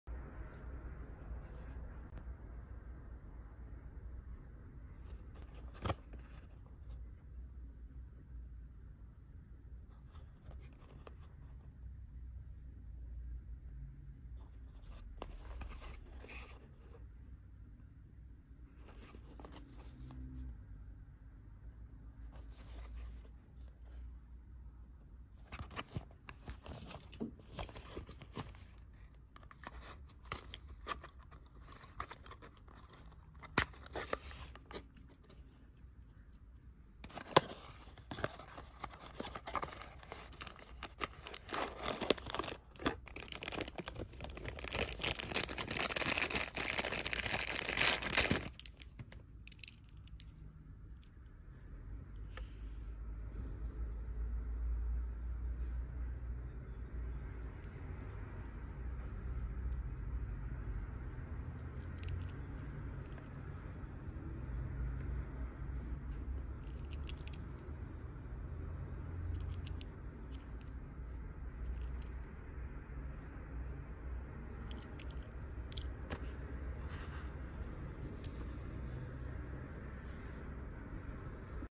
No.160 ドリームトミカ 初音ミク 開封
初音ミクのドリームトミカです
可愛らしいトミカですね
運転してるミクがﾐｸﾀﾞﾖｰに見えるのは気のせいでしょうか